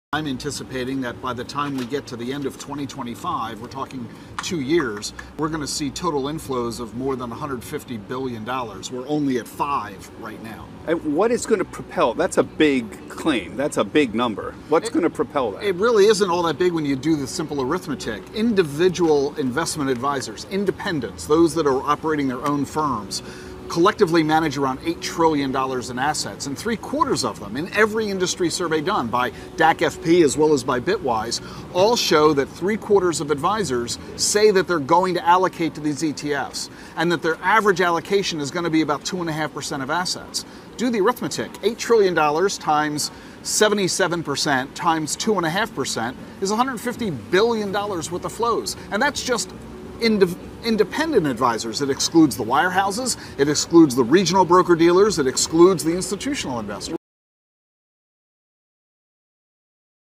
Bitcoin Boom: Advisors Eyeing $150B Inflows!
Why 2025 will be a monumental year for Bitcoin!